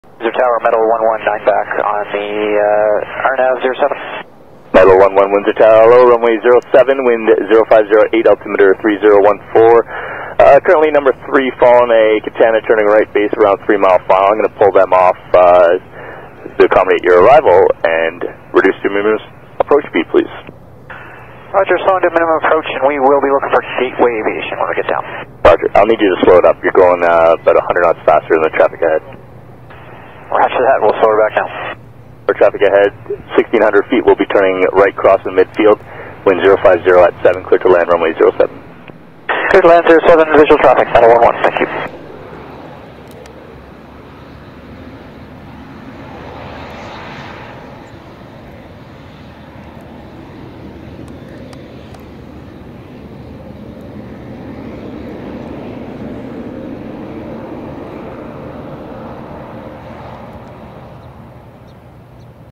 🔴 NEW RCAF | CE-145C VIGILANCE (MAISR) 🔴 Arriving At Windsor International Airport As METAL11.
The RCAF recently received the keys to their new CE-145C Vigilance￼, Manned Airborne Intelligence, Surveillance and Reconnaissance aircraft (MAISR). The CE-145C is built on the 2020 Beechcraft 350ER King Air frame. They were nice enough to show it off on arrival into Windsor, Ontario On March 7th, 2024. REG#145404

*Please note the ATC Audio is not chronographically correct. It may be off by a few seconds, it’s meant to give you an idea of what’s going on.*